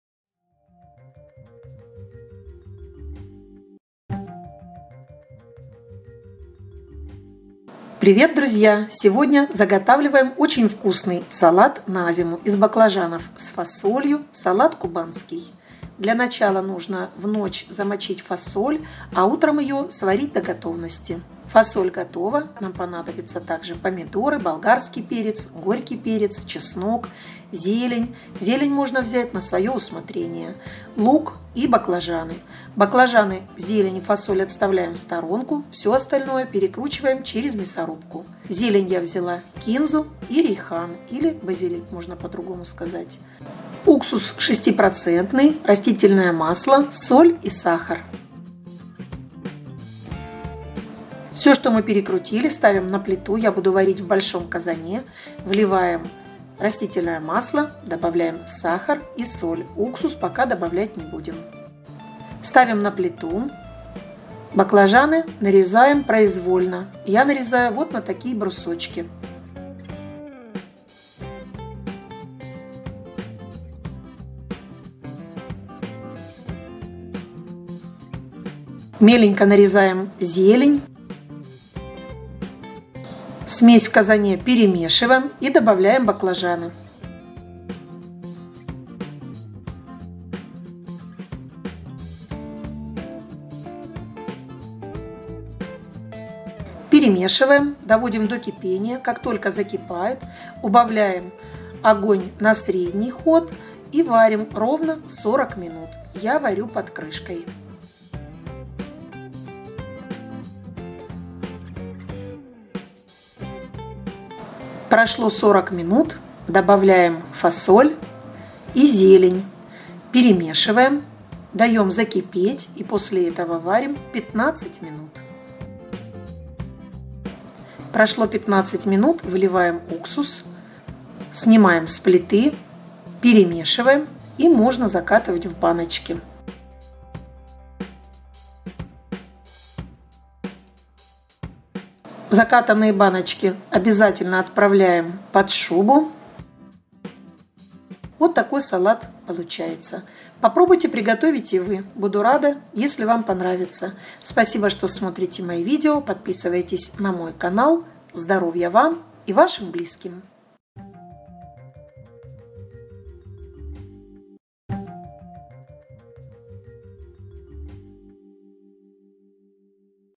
Салат "КУБАНСКИЙ"- из баклажанов и фасоли
РЕЦЕПТ НАПИСАН НИЖЕ

РЕЦЕПТ:

баклажаны 2 кг
фасоль 500 гр
болгарский перец 500 гр
лук репчатый 600 гр
чеснок 2 головки
помидоры 2 кг
горкий перец 2 шт(по желанию)
соль 2 ст.л. с горкой
сахар 150 гр
растительное масло 1 стакан
уксус 6% 100 мл
зелень,любую по вкусу,я брала рейхан и кинзу.